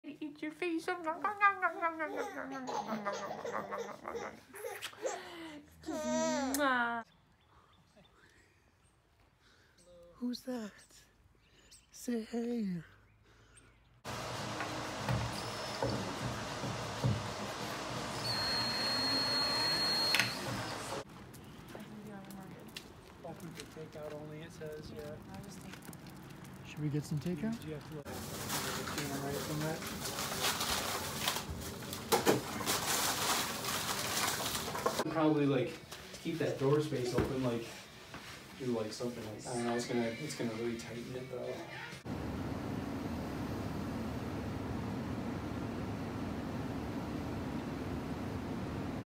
7x7 - Week of Mar 22nd, 2020
1) Kiss attack. 2) Coming out to welcome Nick. 3) Working on things around the house. 4) Out for a walk. 5) Unloading groceries. 6) Setting up Nick's room. 7) In the office with Imogene napping by her favorite fireplace.